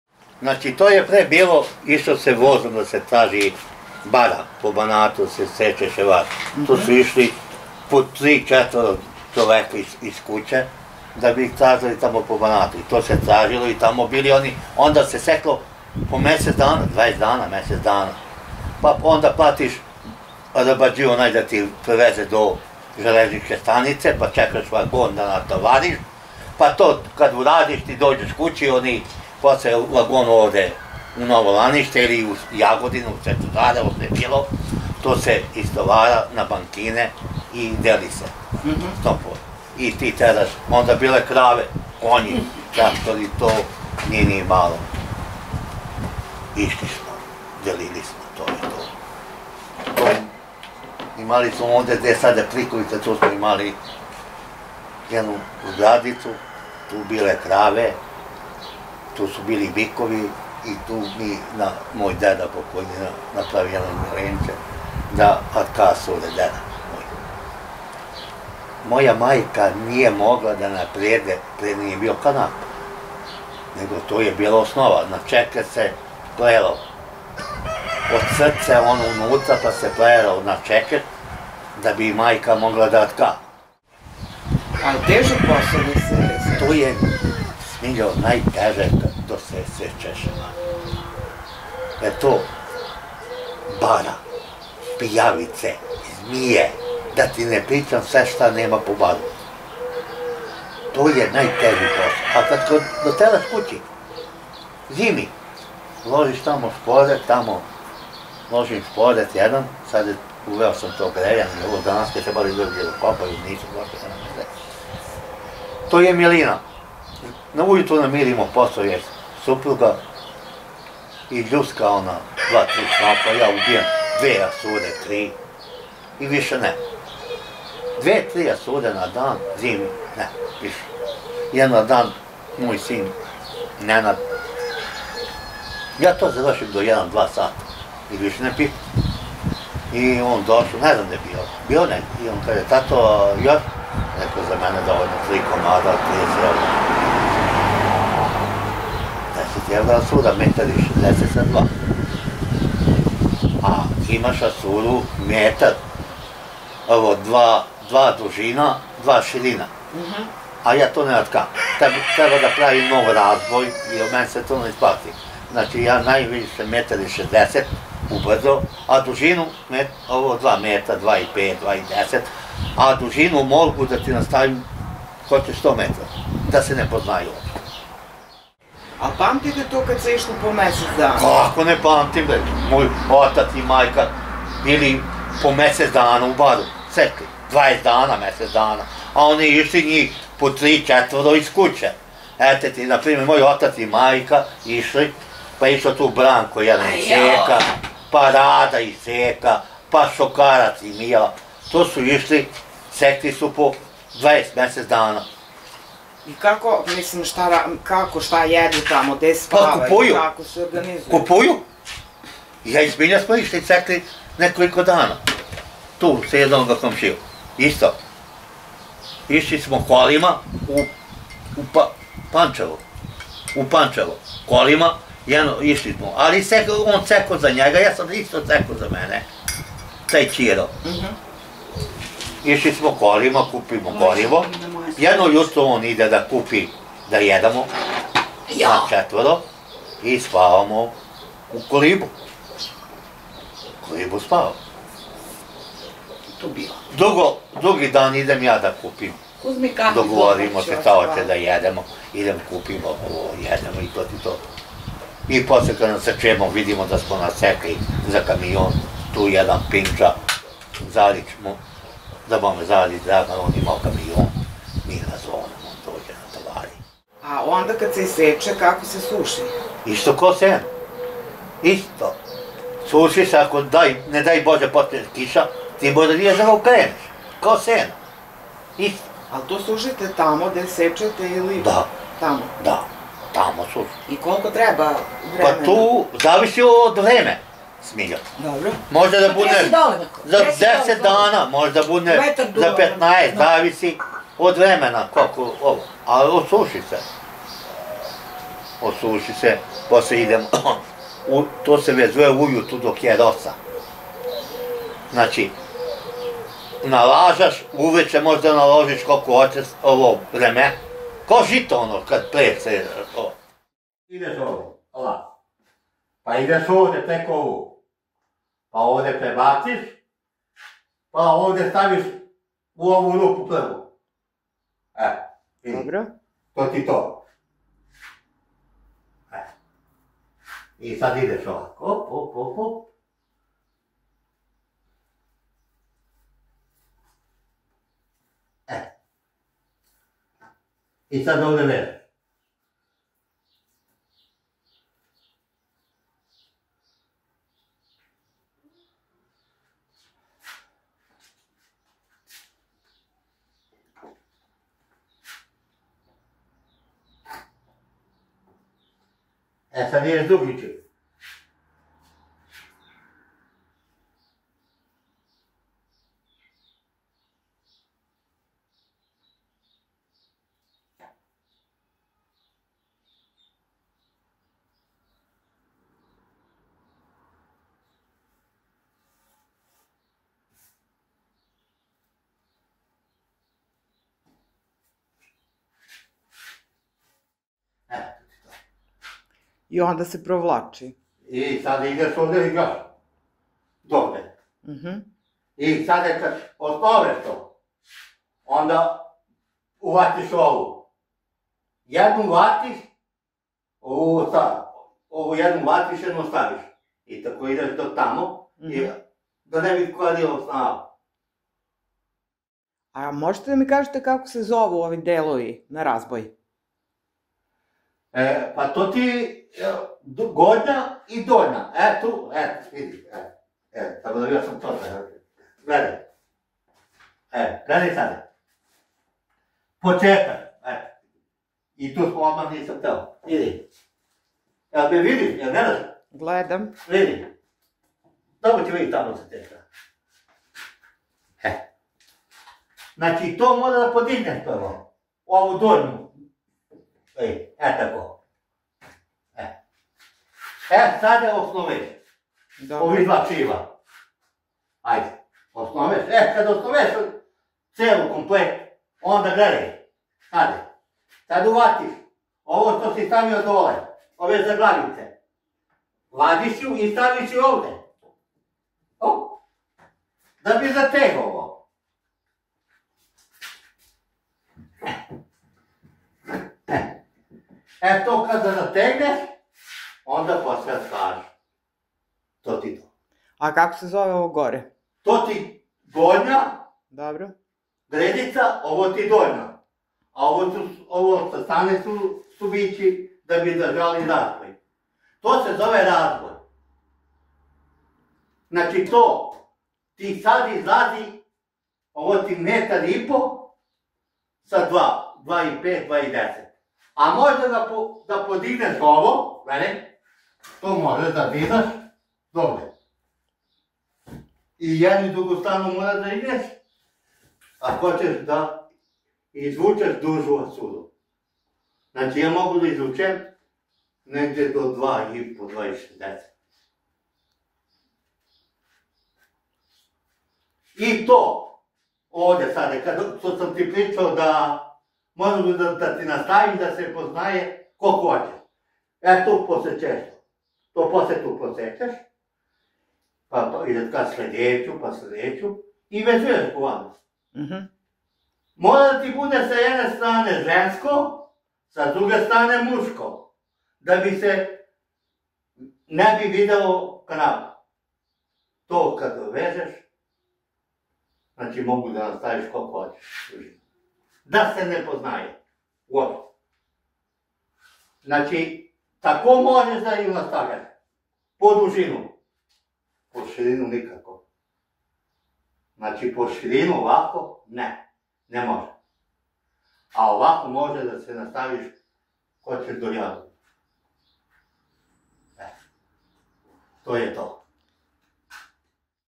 Asurdžijski zanat: Staro Lanište
Nematerijalno kulturno nasleđe Srbije: stari zanati
Asurdžijski zanat, Radiša Marković, Staro Lanište, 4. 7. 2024.
Snimak i montaža: Smiljana Đorđević Belić